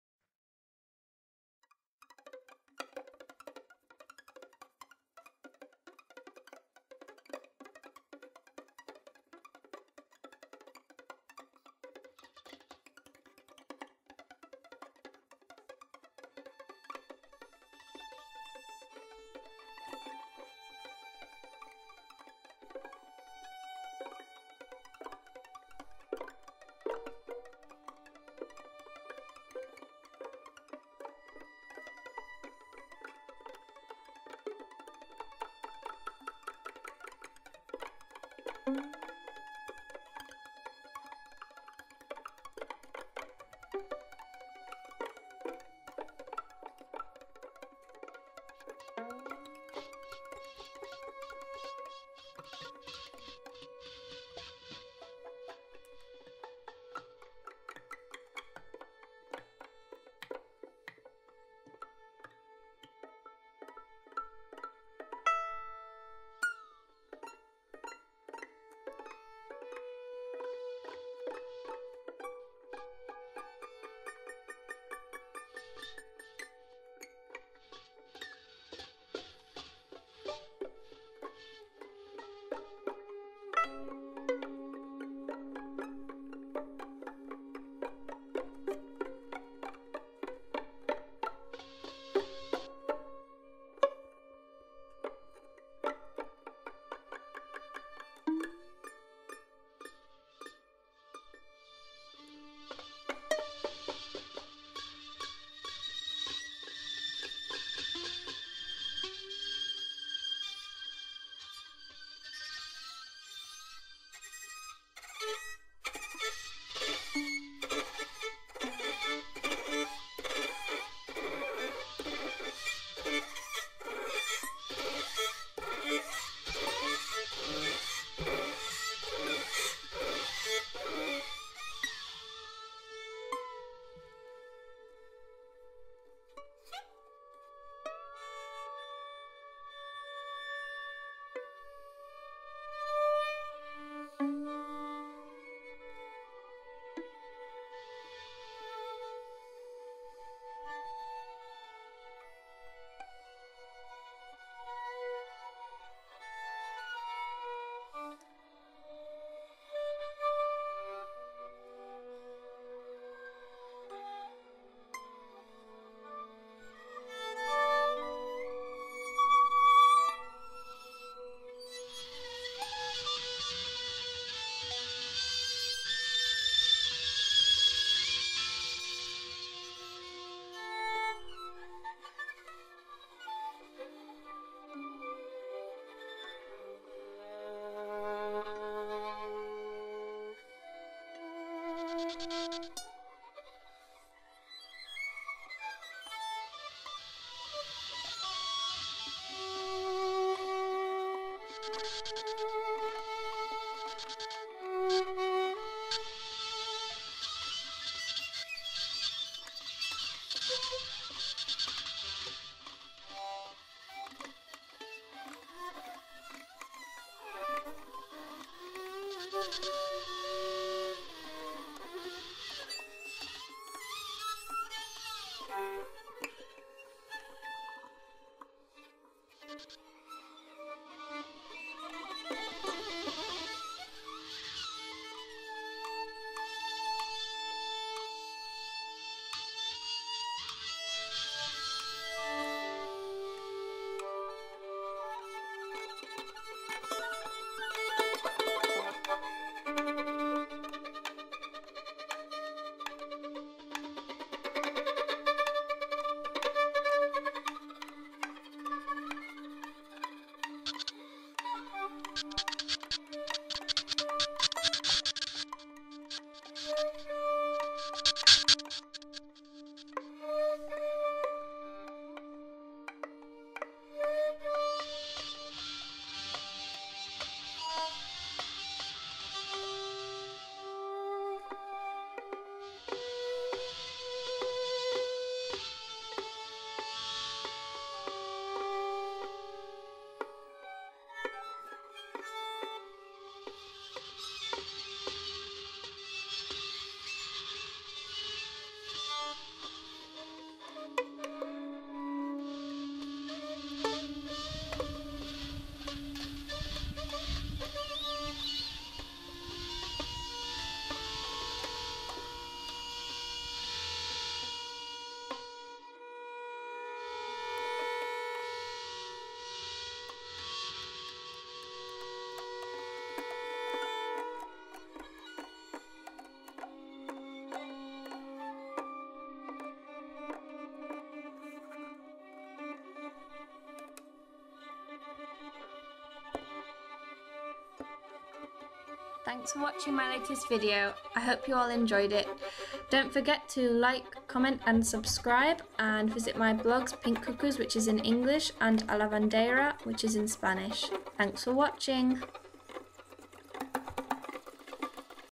Last visit to Chinchilla September 2014 // PINK CUCKOOS
PINK CUCKOOS

One last visit to the Sierra de Chinchilla before flying home...